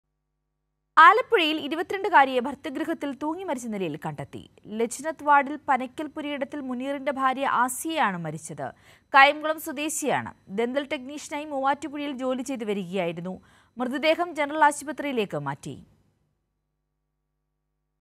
ഭർതൃഗൃഹത്തിൽ 22 കാരിയെ തൂങ്ങി മരിച്ച നിലയിൽ കണ്ടെത്തി | ALAPPUZHA
ആലപ്പു‍ഴയിൽ  ഭർതൃഗൃഹത്തിൽ  22 കാരിയെ തൂങ്ങി മരിച്ച നിലയിൽ  കണ്ടെത്തി
A 22-year-old woman was found hanging dead in her husband's house